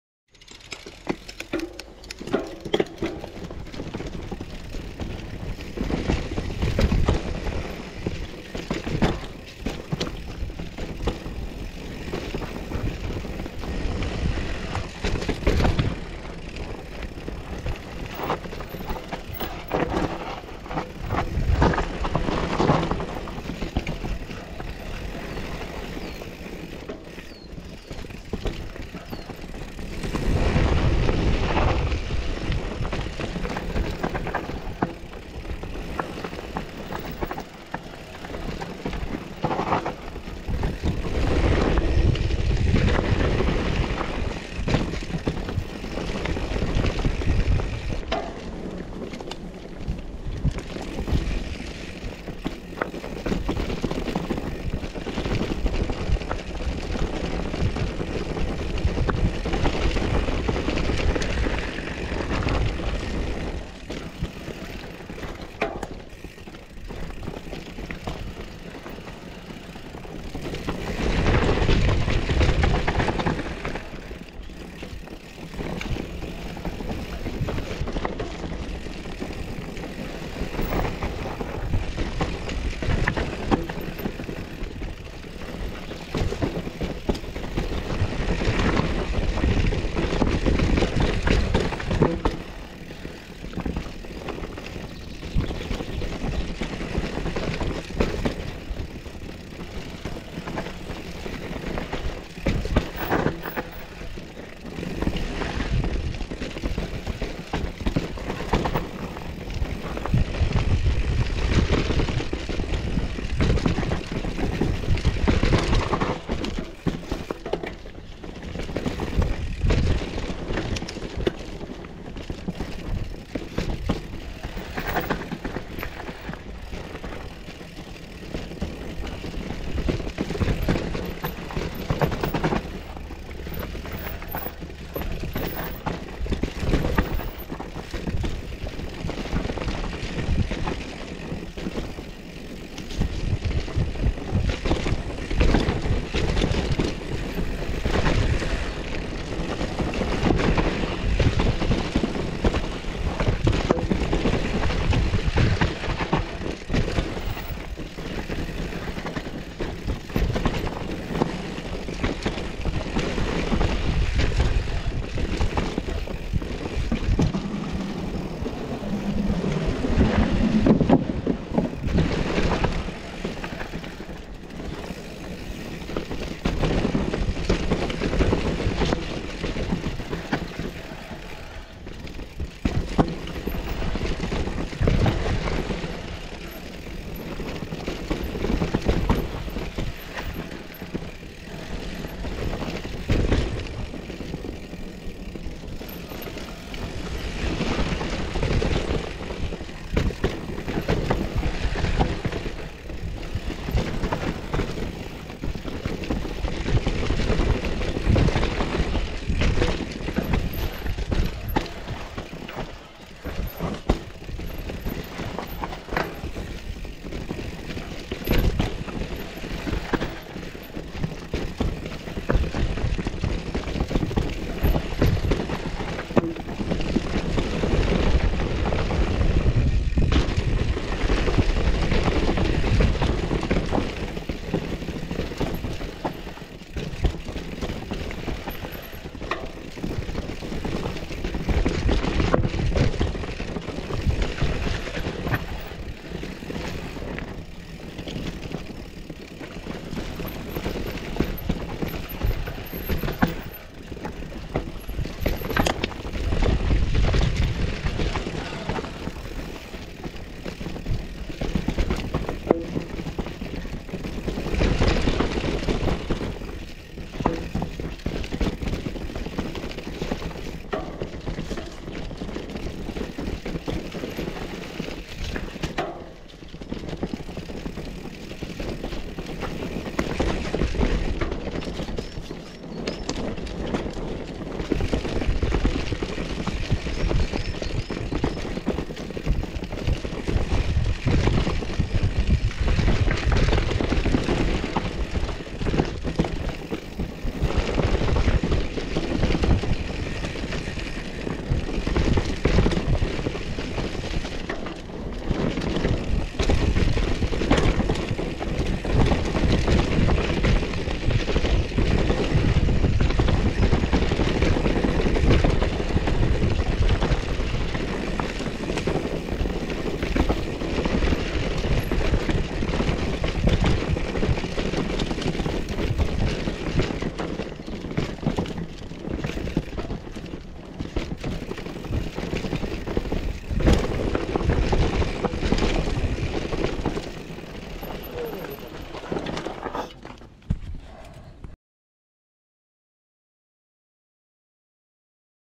Zbój (czerwony) - Bike Park Szczyrk - 2024
Czerwona naturalna trasa o długości ok. 1,5 km. Zaraz za nią zaczyna się Otik, co w sumie daje ok. 4 km ciekawego zjazdu.